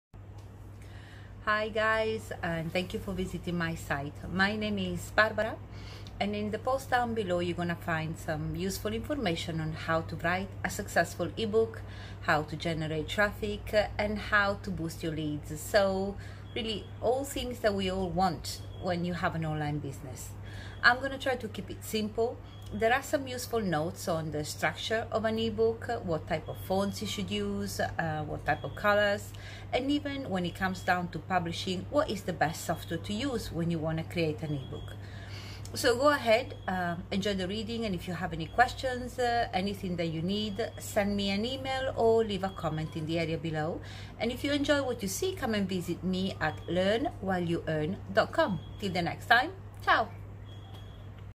How To write A Successful EBook
A small video introduction to a post I wrote with tips and suggestions on how to write an EBook and create a lead magnet for your business.